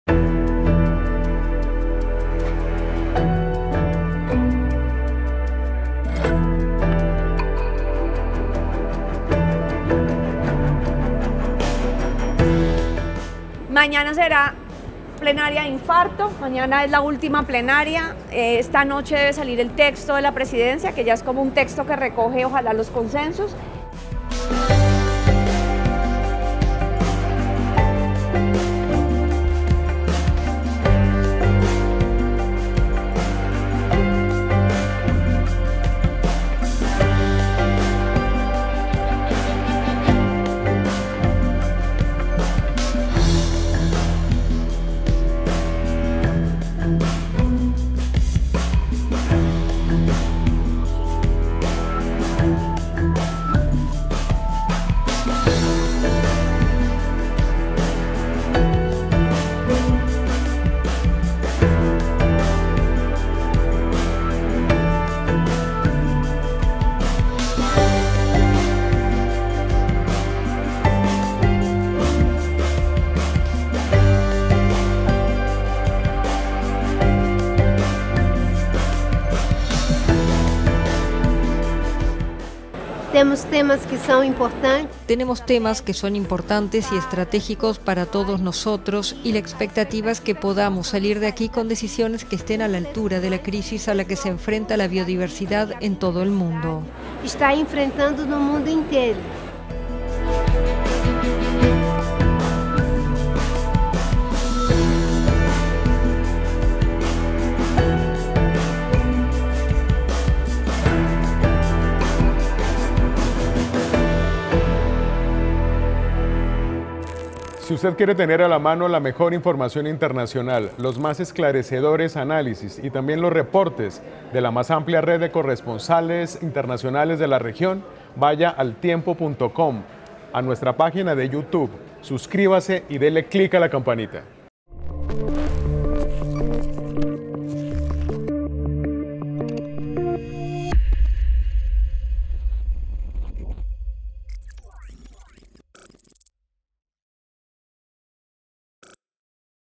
Aumenta la tension en la recta final de negociaciones de la COP16 en Colombia | El Tiempo
Las tensiones en las negociaciones sobre cómo frenar la devastación de la naturaleza en la COP16 hacen prever un final "de infarto" en el tema de la financiación, afirma Susana Muhamad, presidenta de la COP16 y ministra de Medio Ambiente de Colombia. IMÁGENES Y SONIDOS